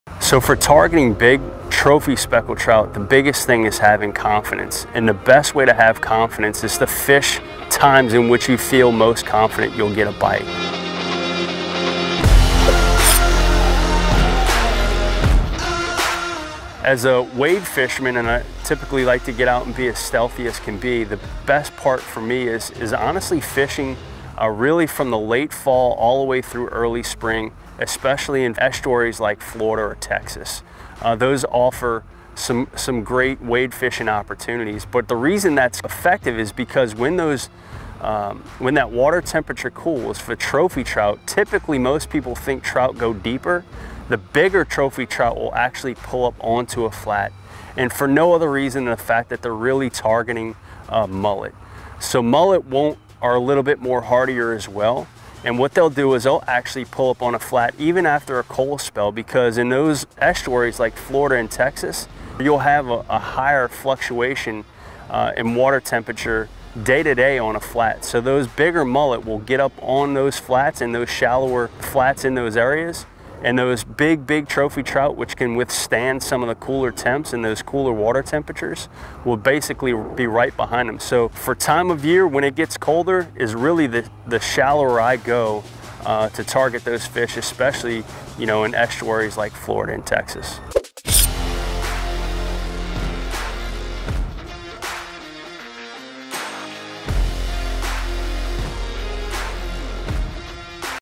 Best Time of Year to Catch Big Speckled Trout
As a wade fisherman, fishing late fall to early spring is ideal especially in estuaries in Florida and Texas. When the water cools, trophy trout will pull up onto a flat chasing the heartier mullet. The water temperature fluctuates more in those flats day to day. When the weather gets colder, go shallower.